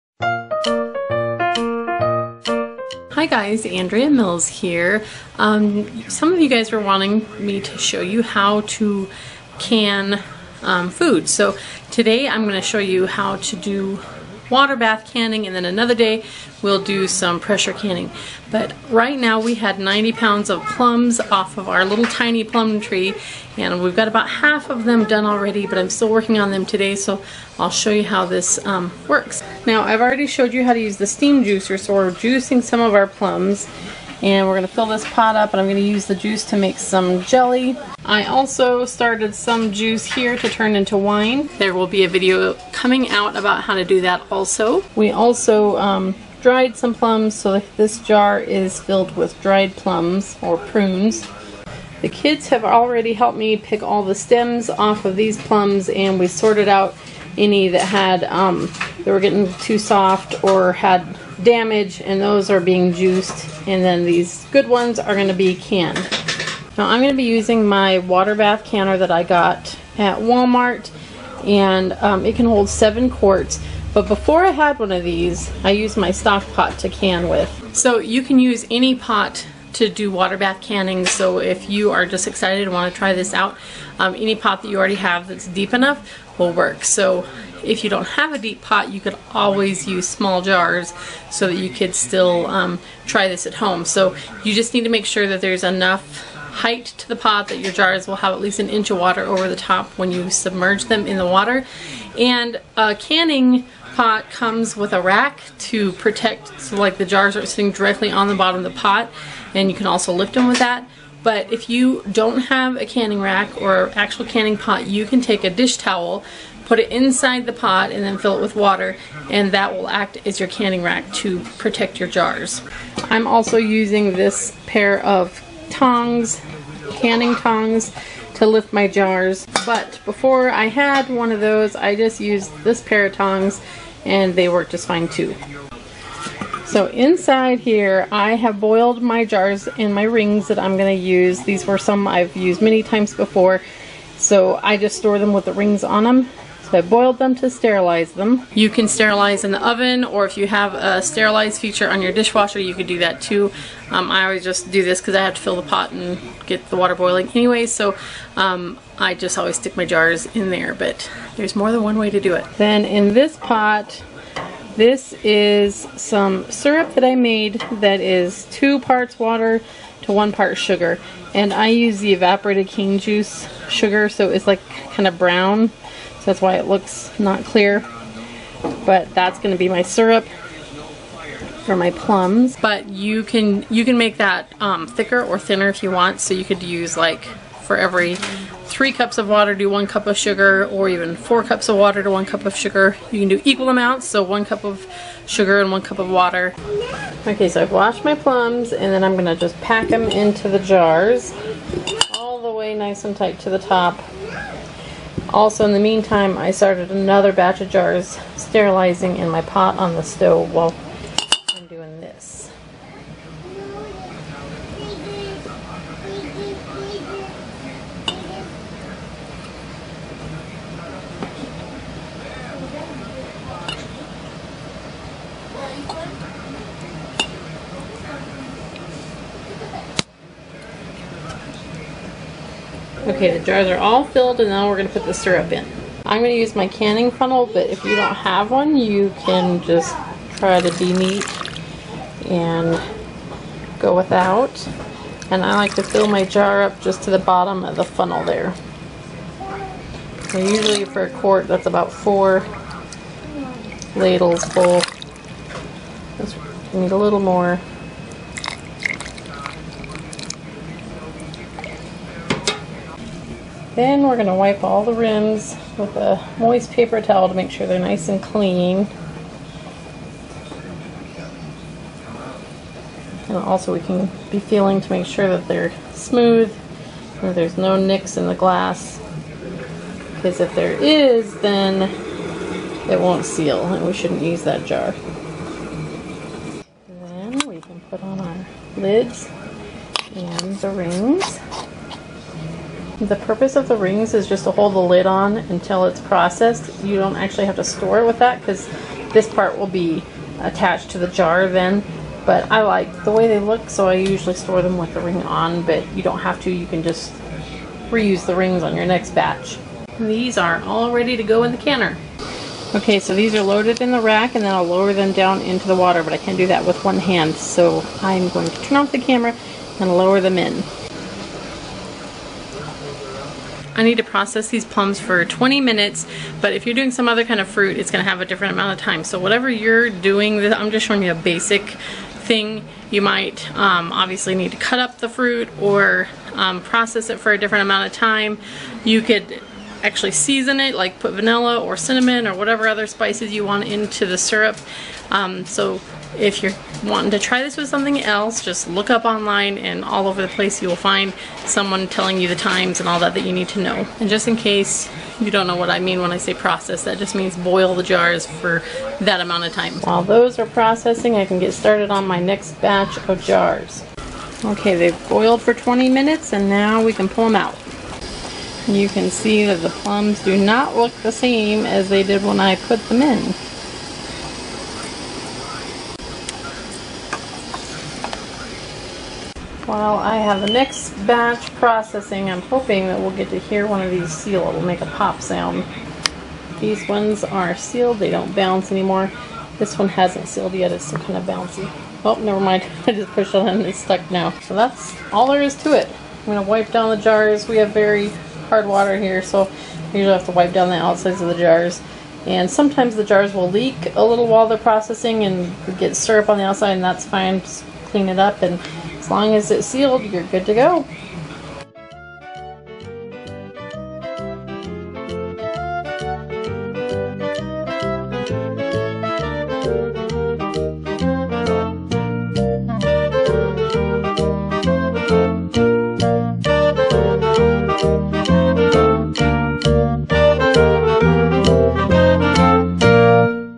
Basic Water Bath Canning ║ Plums │Food Preservation
Water bath canning is suitable for fruits and tomatoes only. Don not water bath can vegetables or meats. In this video I demonstrate the basic steps to water bath can fruit in syrup. I canned whole plums, but the same basic method can be used for any kind of fruit. Consult a canning chart to get specific processing times for the type of fruit you're using.

Mills Family
Moorcroft, Wyoming 82721
USA